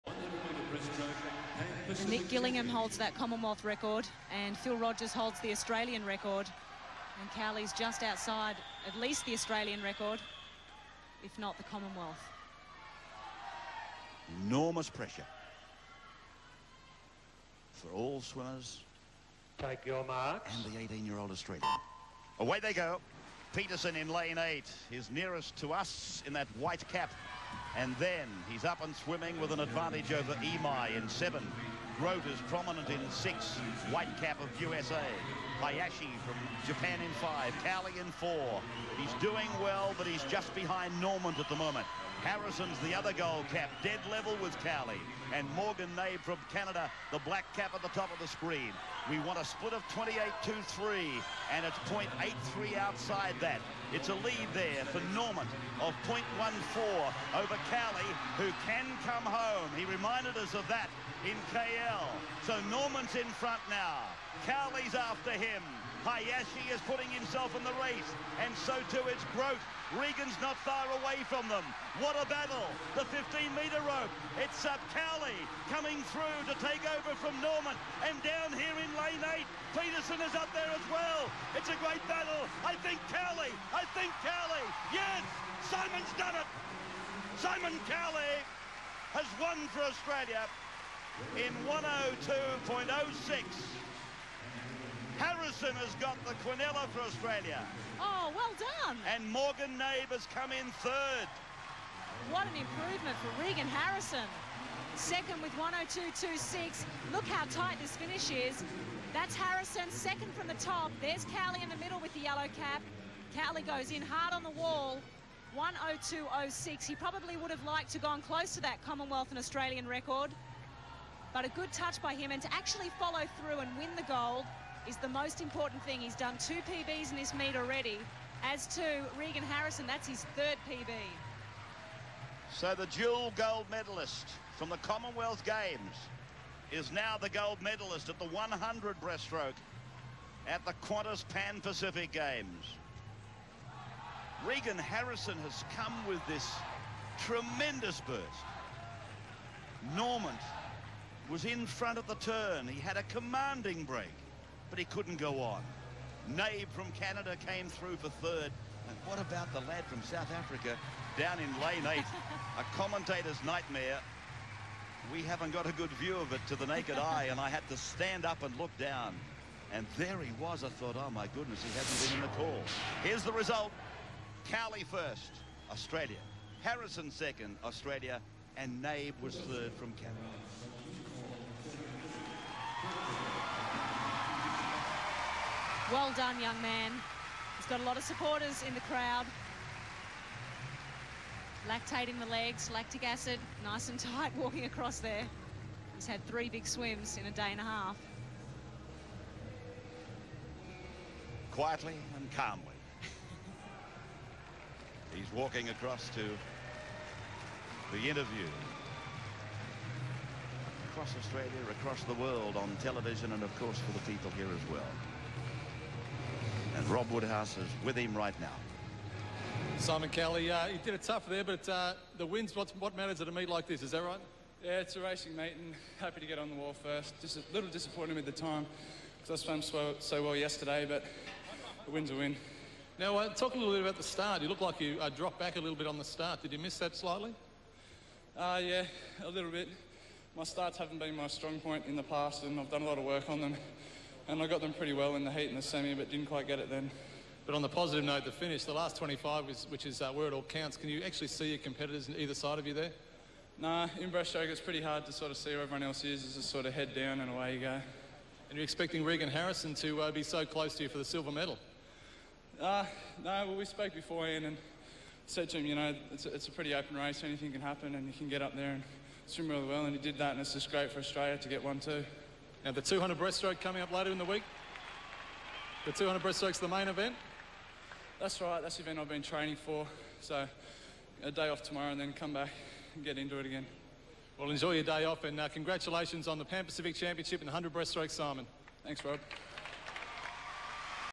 1999 | Simon Cowley | Pan Pacific Champion | 100m Breastroke | 1.02.06 | 18 Years Old
Australian Simon Cowley winning the 100m Breastroke at the 1999 Pan Pacific Championships in Sydney | He sadly missed selection for the Sydney Olympics the following year | This footage has been taken from a Fox Sports Australia broadcast | If this is a breach of copyright please advise and it will be removed |